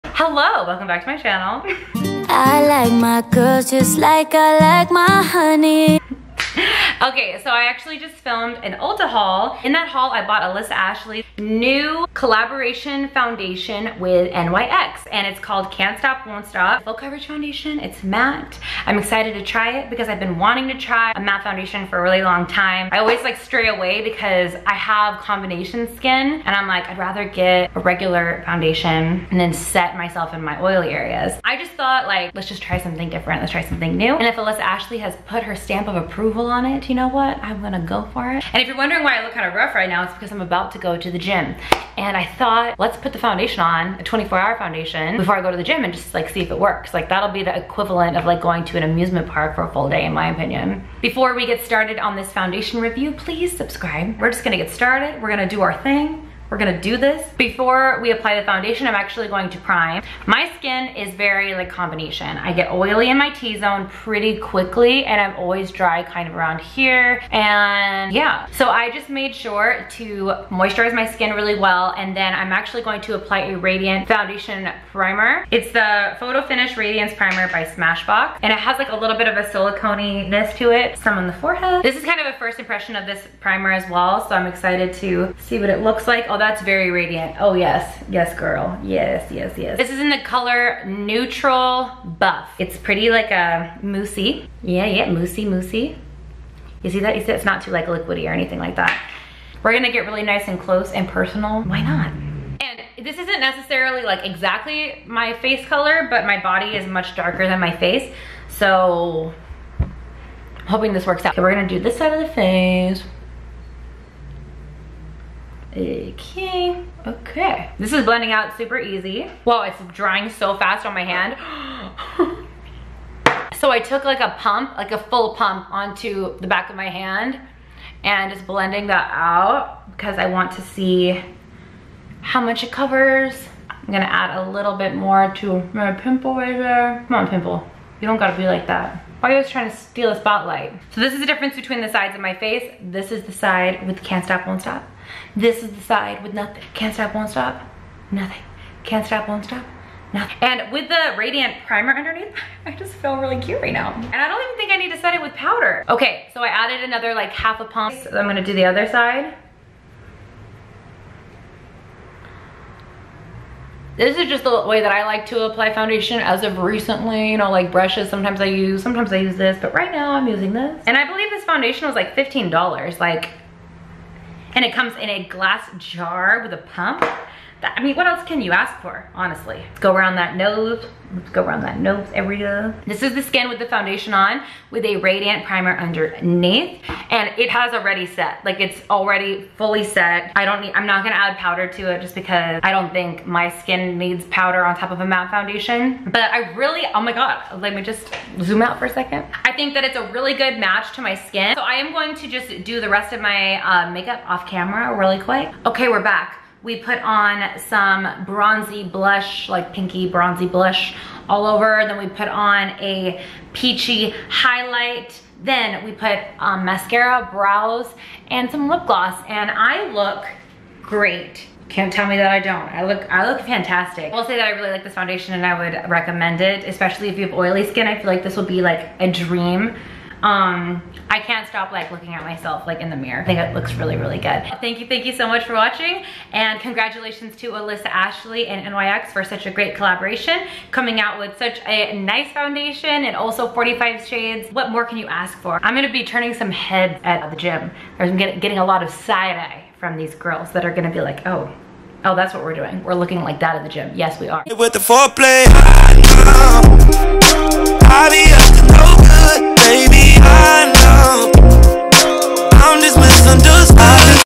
ALISSA ASHLEY & NYX DID THAT!!! | CAN'T STOP WON'T STOP FOUNDATION REVIEW
I tried Alissa Ashley's collaboration with NYX! I haven't tried many matte foundations, but I am literally obsessed with this one. It is a matte foundation with great coverage. I also love Alissa Ashley and how great she is with makeup. I started watching her channel from her hooded eyes tutorials!

It has 45 shades, great packaging (WITH A PUMP), and great coverage.